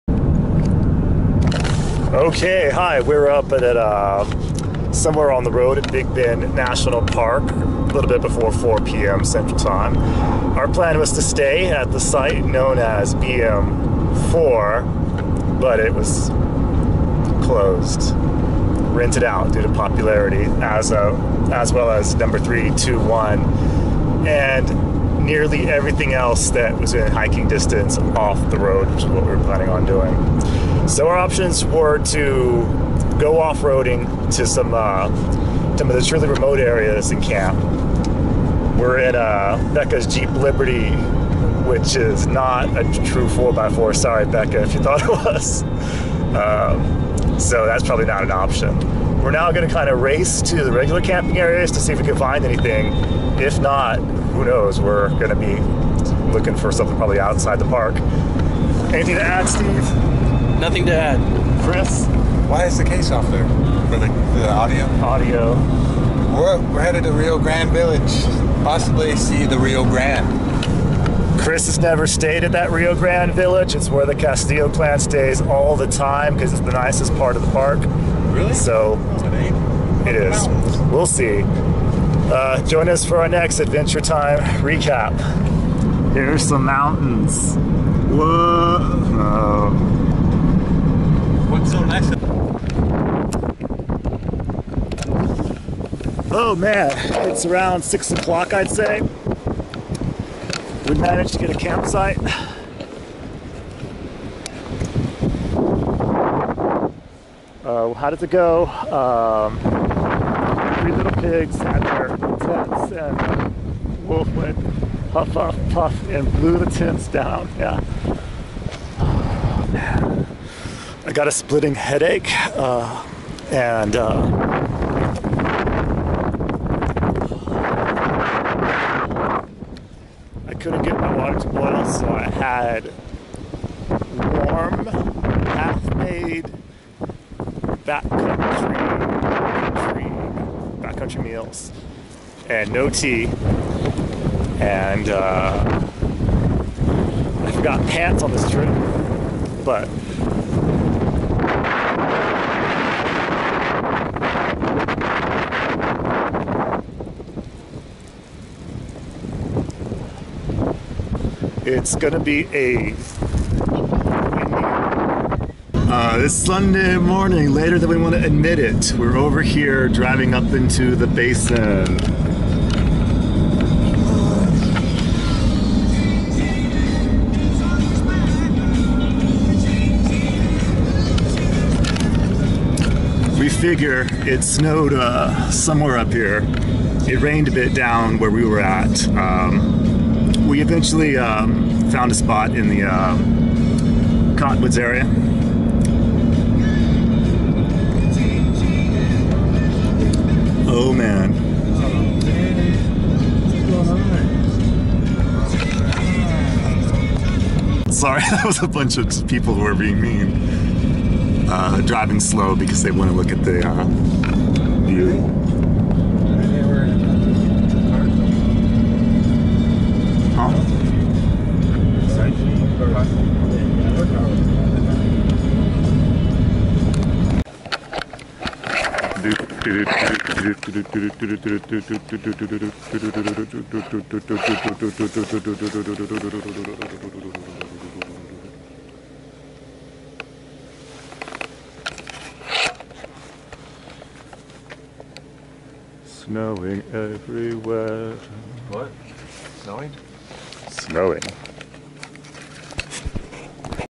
Big Bend Travel Log
Big Bend 2014 trip travel log for night one